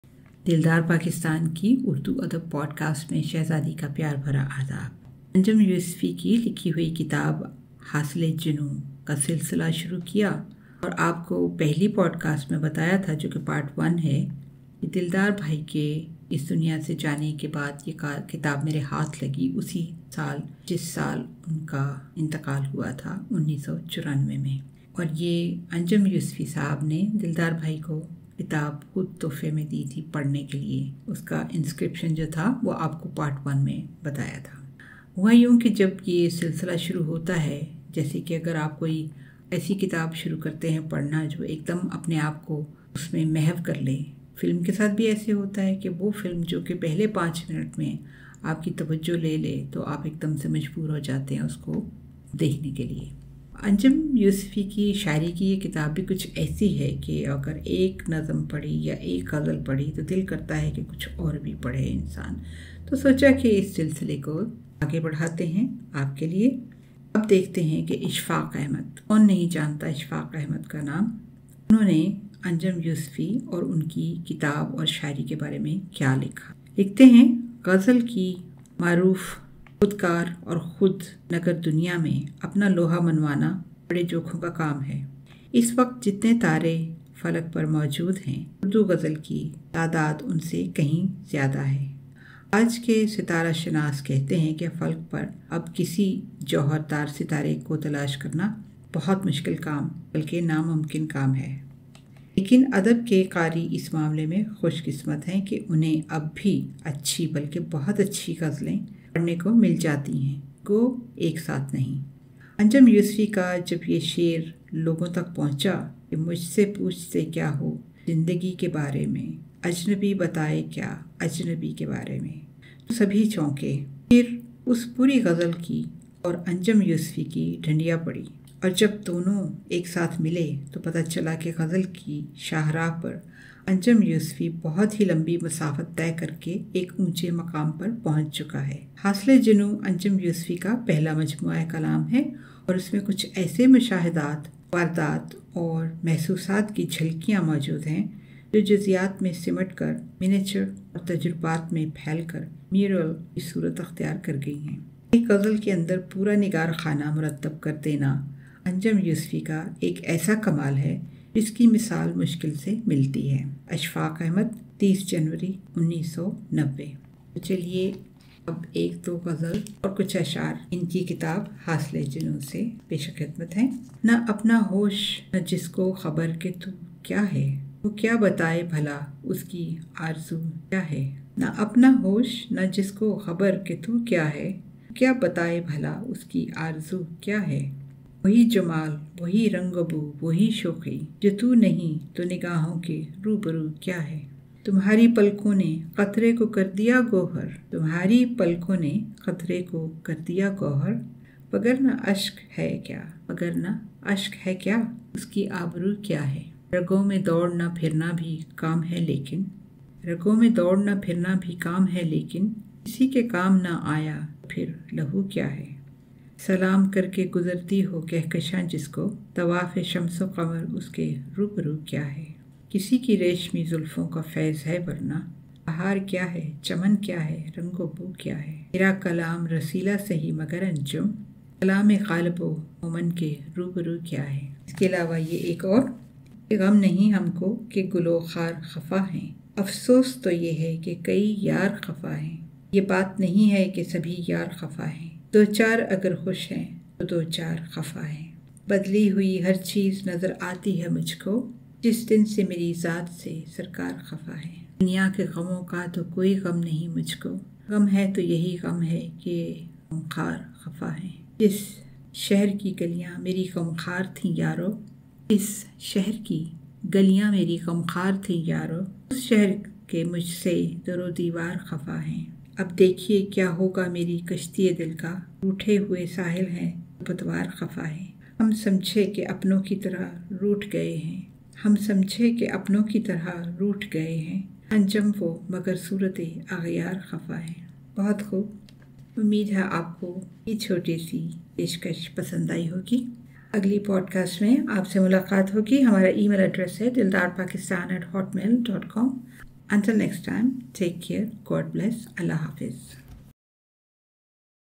HASIL-E-JANOON_PART_3
Urdu is the most beautiful language to listen to even if you can't understand it fully. Urdu Literature has unlimited treasure of poetry and prose.

 Beautiful forms of poetry has been written by many gifted and talented poets.

 Some of that poetry has become part of the every day culture due to music, movies, books and recital gatherings.

 Anjam Yusfi's book, Hasil-e-Janoon is one of the books that I found in my elder brother Dildar Pervaiz Bhatti's collection, after his death.

 I never got to discuss contents of all these books with my brother.

 I will be sharing some poetry and excerpts from both of our collections in future podcasts.